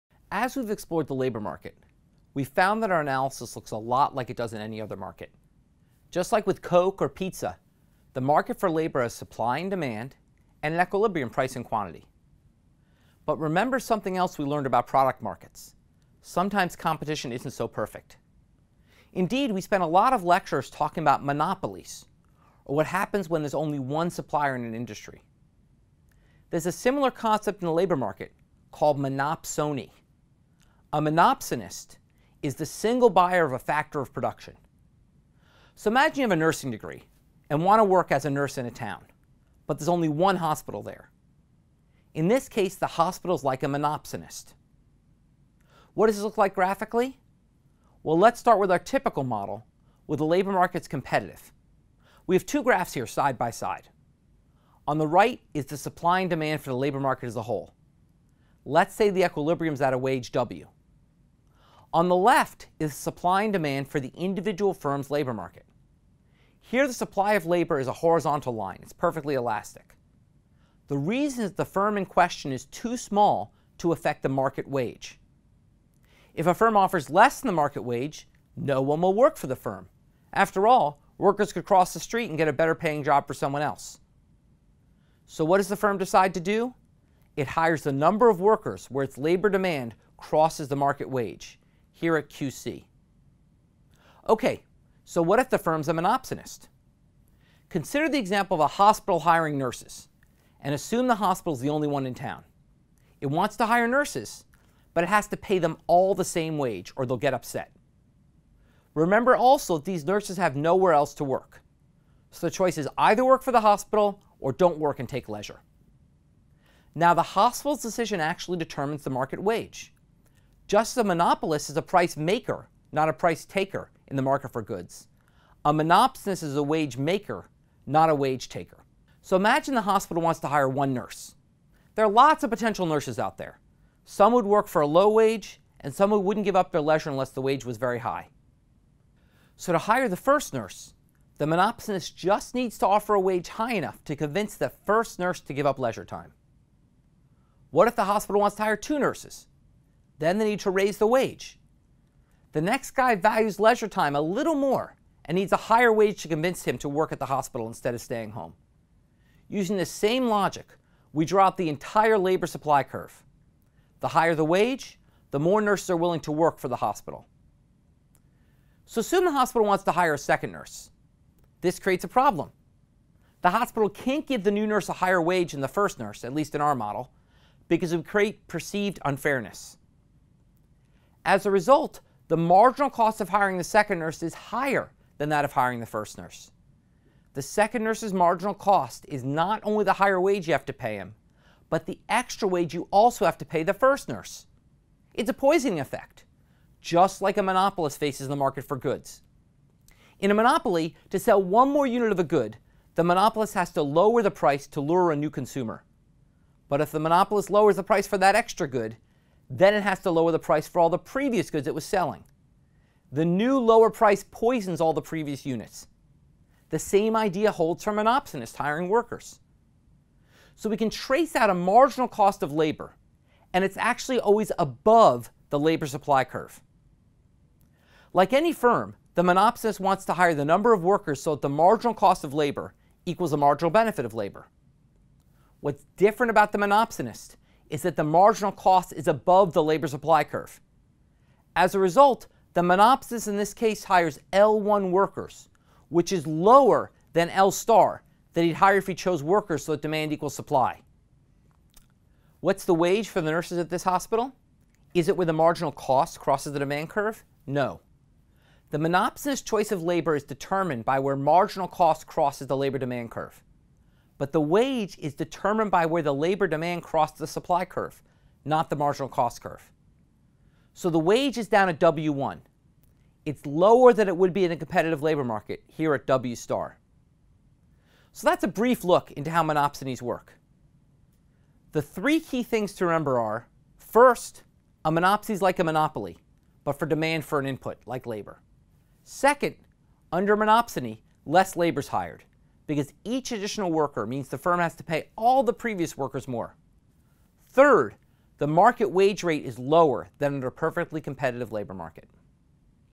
5.6 Monopsony and the Labor Market
What happens when there's only one employer in town?